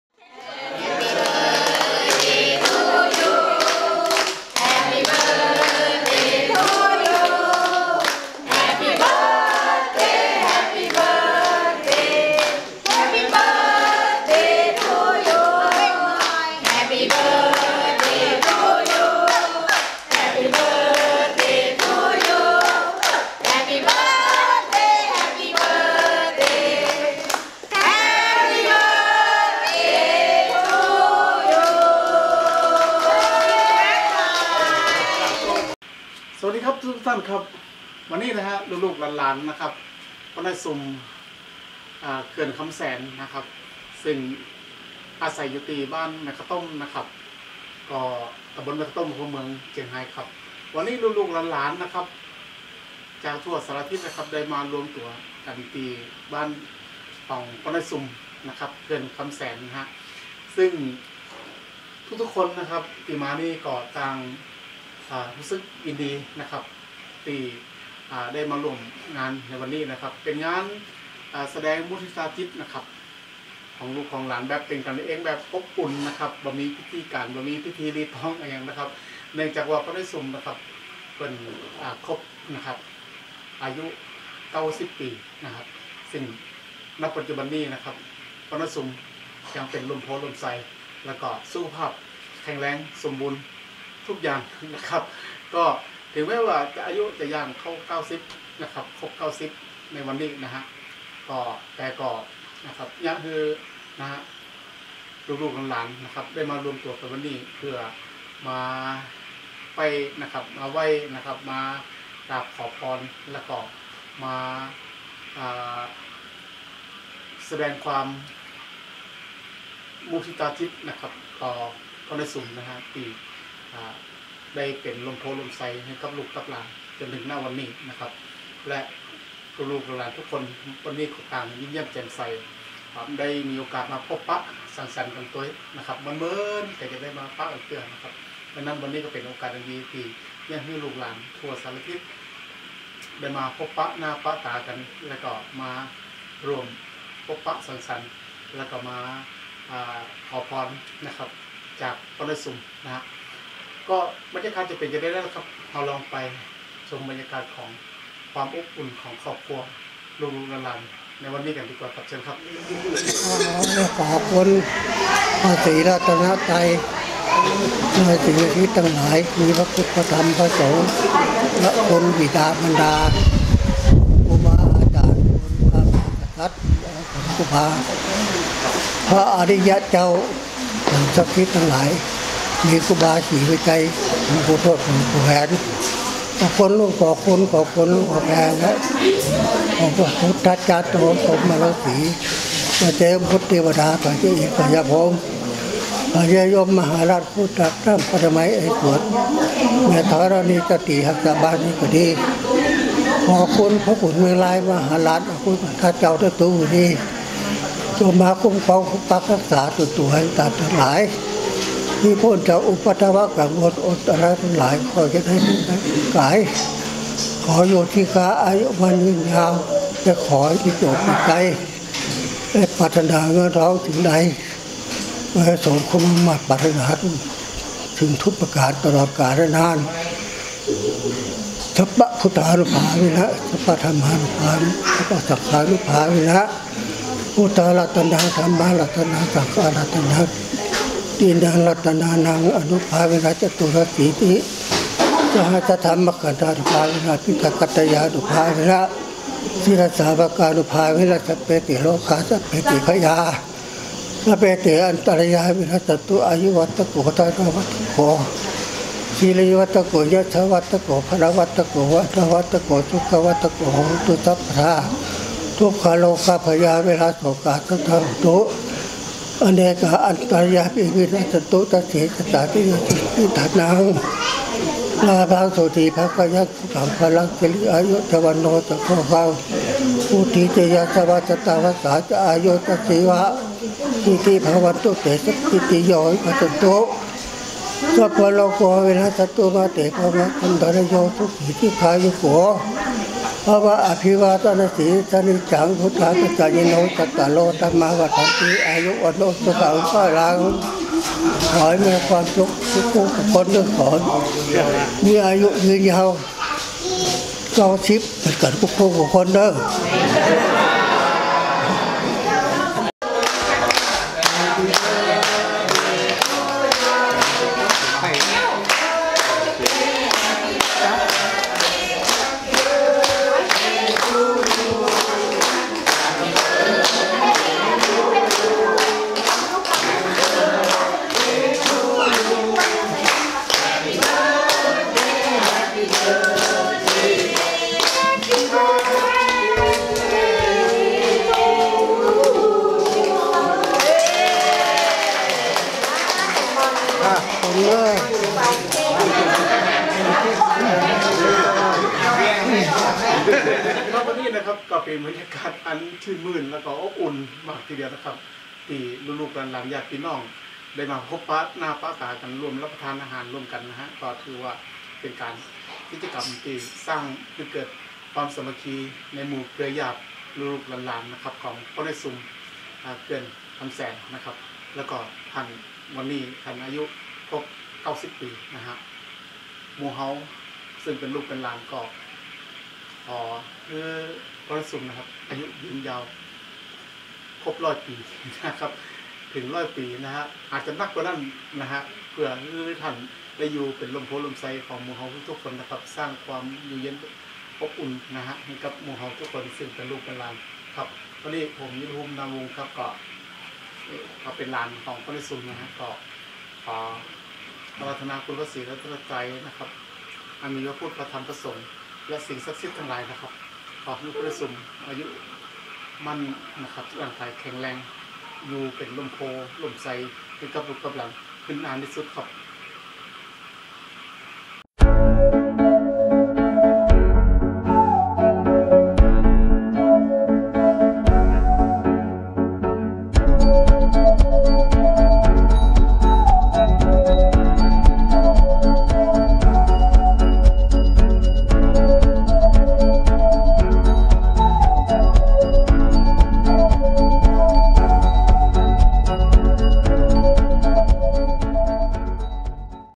90 ปี อายุวัฒนมงคล ของ พ่อน้อยสุ่ม เขื่อนคำแสน
90 ปี อายุวัฒนมงคล ของ พ่อน้อยสุ่ม  เขื่อนคำแสน วันนี้ พุธ 24 กพ.2564 ลูก ๆ หลาน ๆ และญาติพี่น้องของพ่อน้อยสุ่ม ได้มารวมตัวกันอย่างพร้อมหน้าพร้อมตา
เพื่อมาร่วมแสดงมุทิตาจิต และขอพรจากพ่อน้อยสุ่ม เพื่อความเป็นศิริมงคลในการดำเนินชีวิต จากพ่อน้อยสุ่ม เขื่อนคำแสน ในวันมิ่งมงคลนี้
...สร้างความชื่นมื่นและความอบอุ่นยิ่ง..บันทึกเมื่อ วันพุธที่ 24 กพ.2564 เวลา 18.00 น. ณ บ้านแม่ข้าวต้ม ต.แม่ข้าวต้ม อ.เมือง จ.เชียงราย..